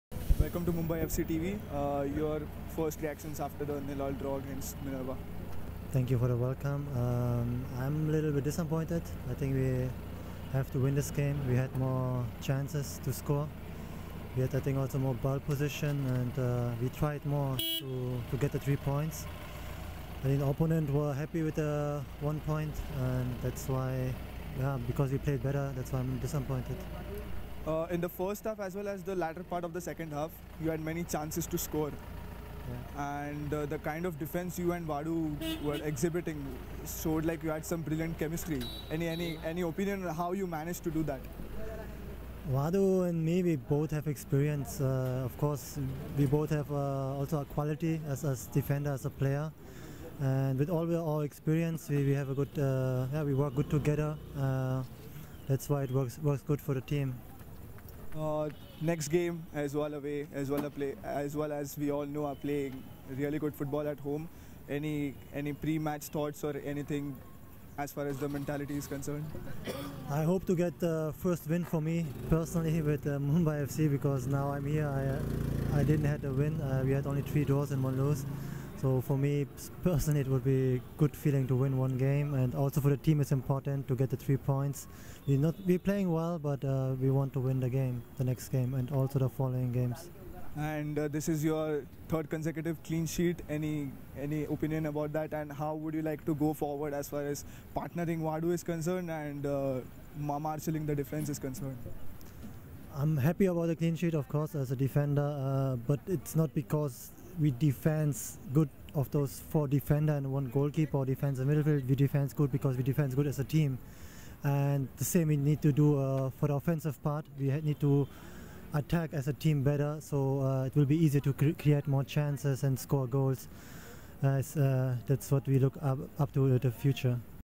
Sharityar : "Winning the away games will be crucial now!"
AmchiMumbai's Djelaludin Sharityar was happy with the clean sheet but not with just one point versus Minerva Punjab FC. Here's his post-match reaction.